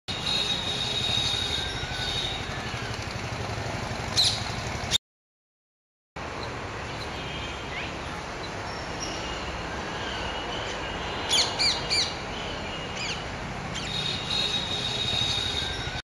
parrot's flute
video taken on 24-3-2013 of a parrot perched on a topmost branch holding a seed with its legs and takes it to its beak as a flute and flies off leaving it to the video grapher